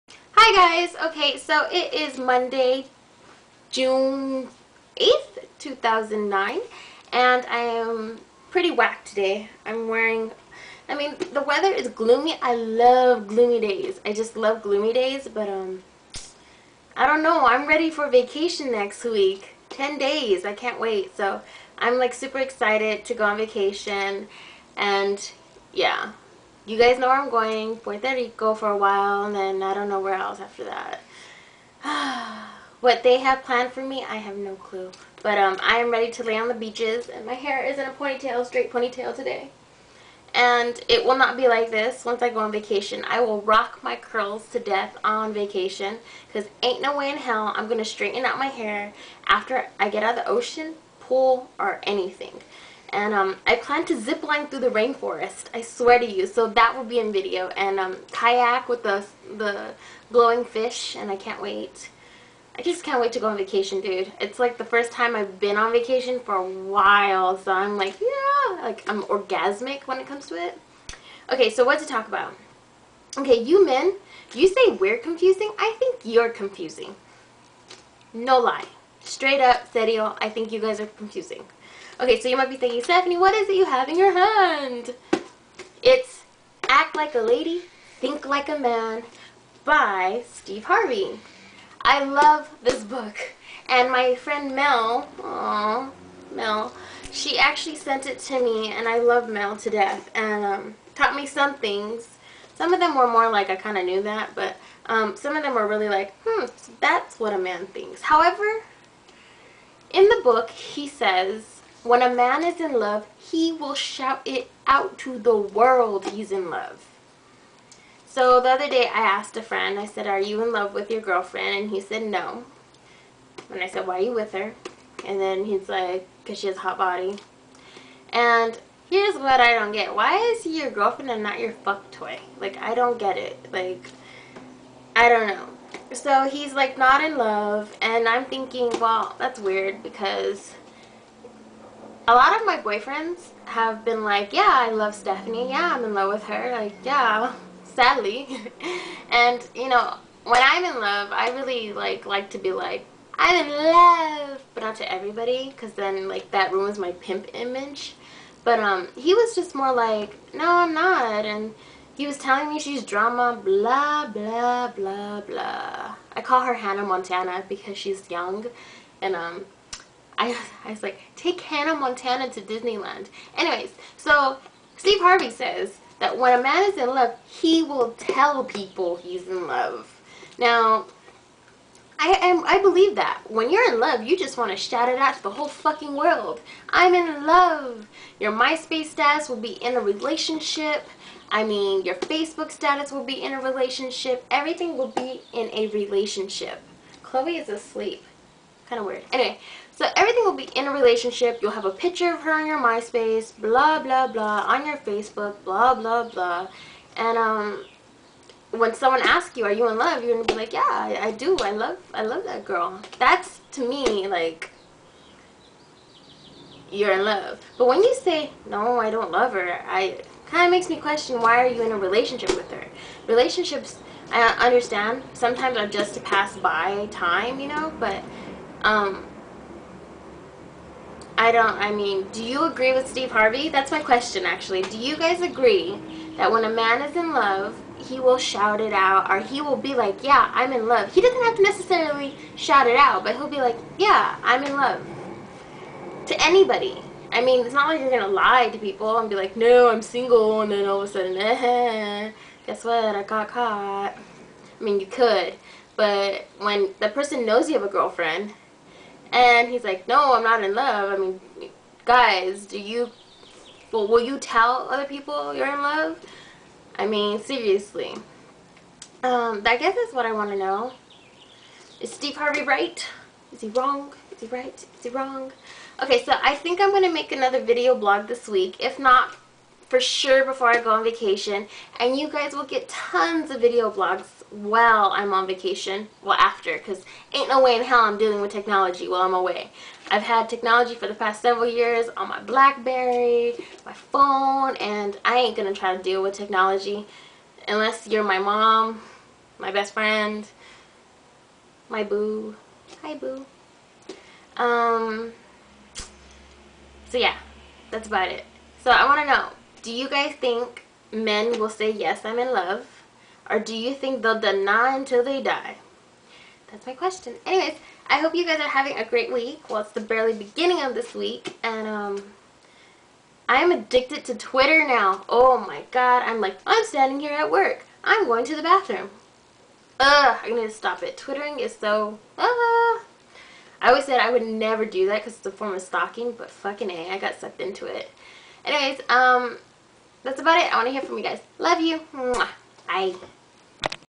VLOG: Do men profess thier love?
Men, do you tell someone your in love when they ask?